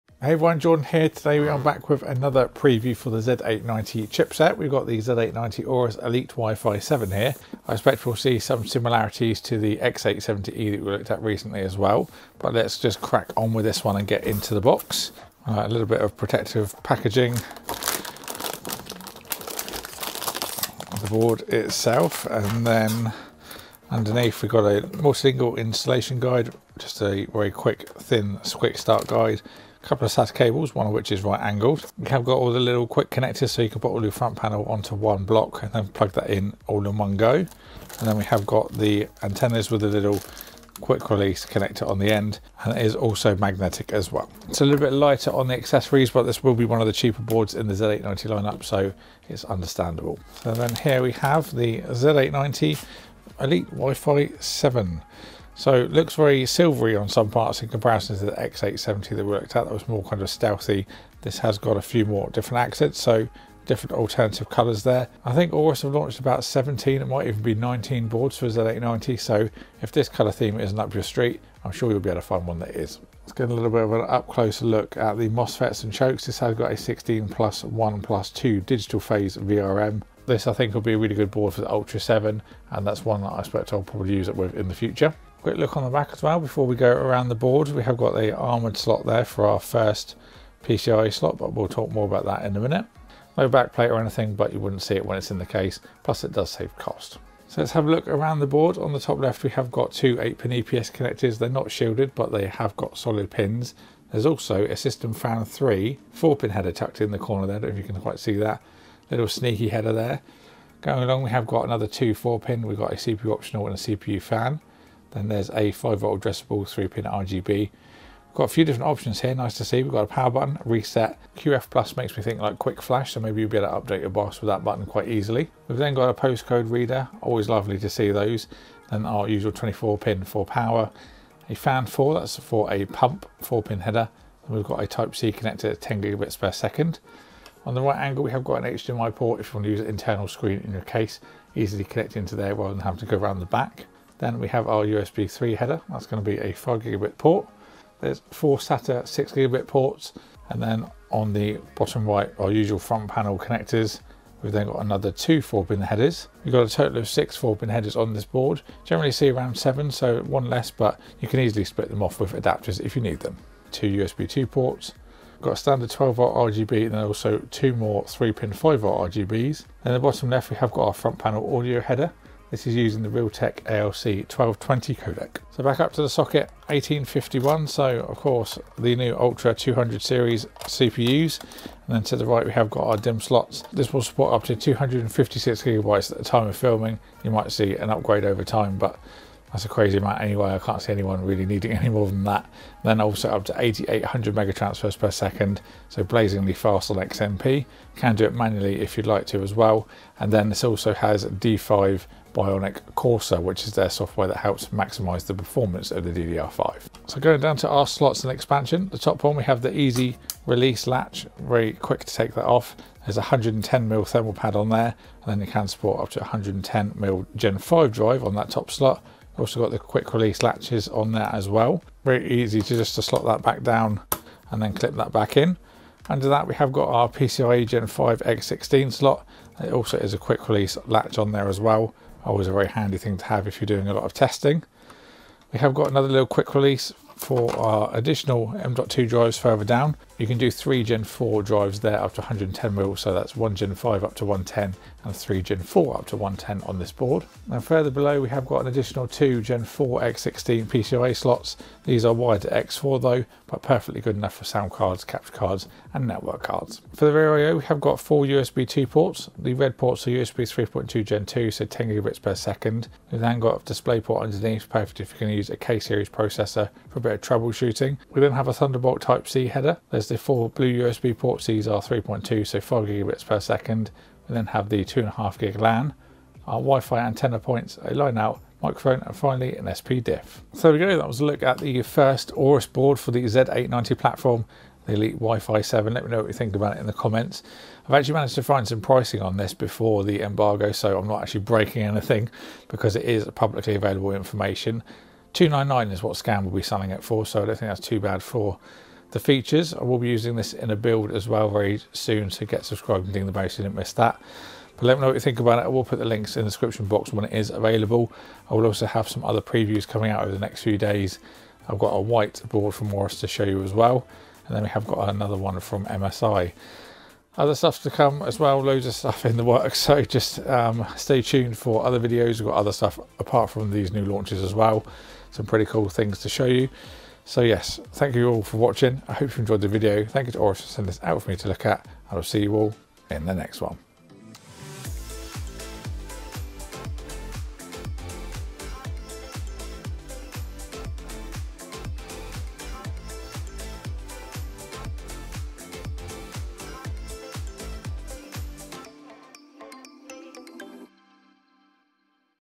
A GREAT VALUE NEXT GEN BOARD! - Z890 AORUS ELITE WIFI7 - Unboxing & Overview! [4K]
Today we look at the AORUS Z890 ELITE WIFI 7, one of the more reasonably priced Z890 motherboards!

My Gaming Chair:
Amazon US (Similar)
Amazon UK

Products featured are provided by manufacturers unless stated otherwise.

Part of Jordan Ash Media.